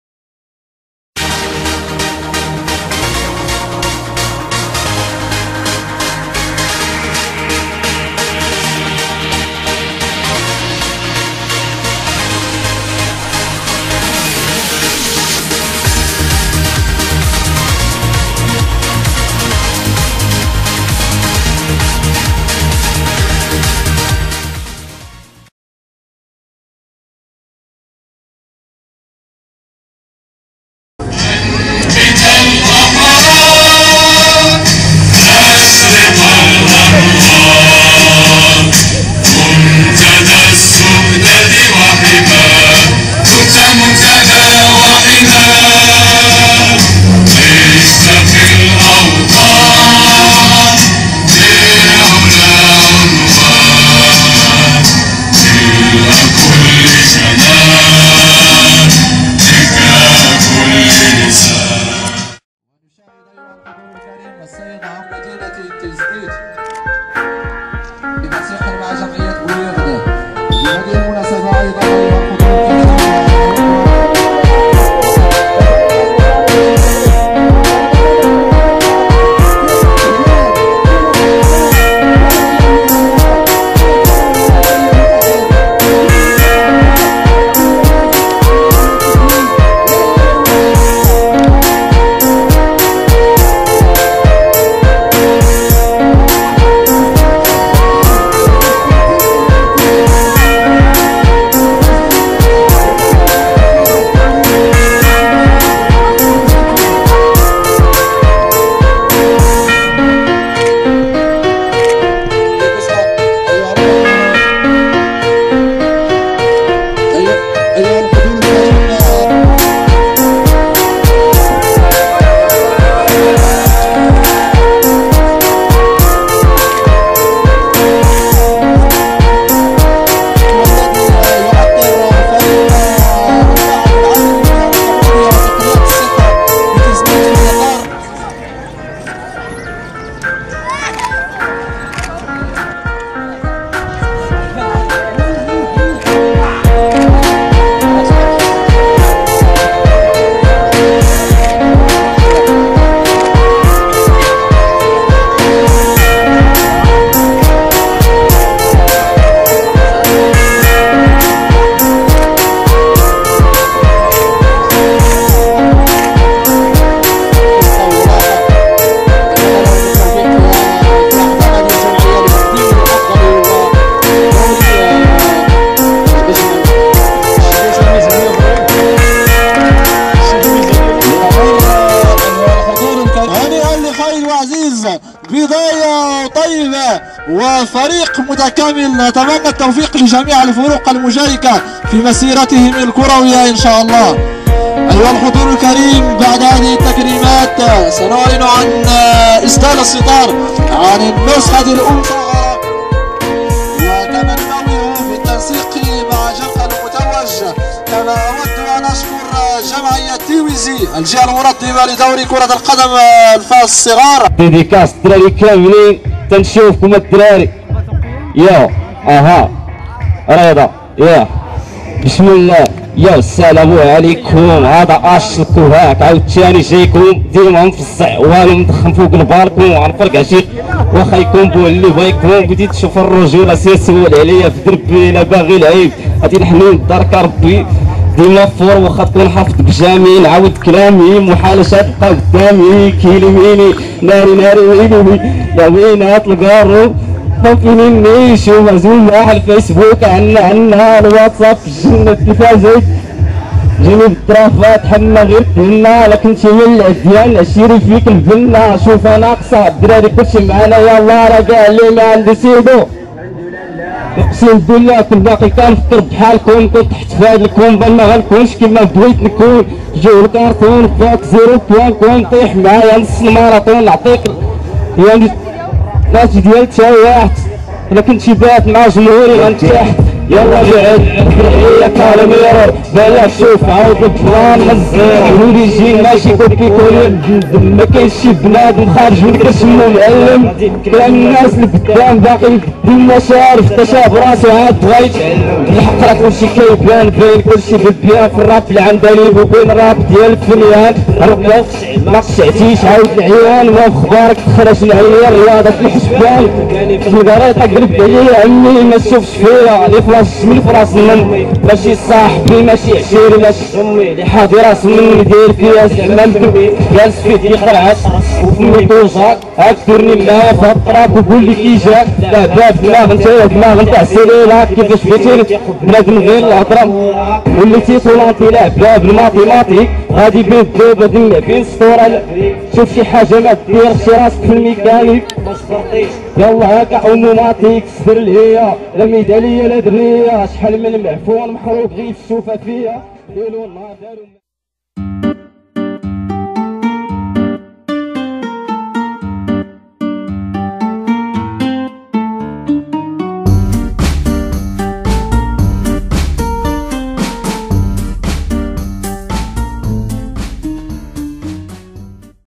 لقطات من الحفل الختامي لدوري جمعية أفاسوفا تيزنيت 2016/04/10
لقطات من الحفل الختامي لدوري كرة القدم المصغرة المنظم لفائدة الشبان من جمعية أفاسوفا للثقافة و الفن و البيئة و الاعمال الاجتماعية بساحة الاستقبال بمدينة تيزنيت بتاريخ 10 أبريل 2016.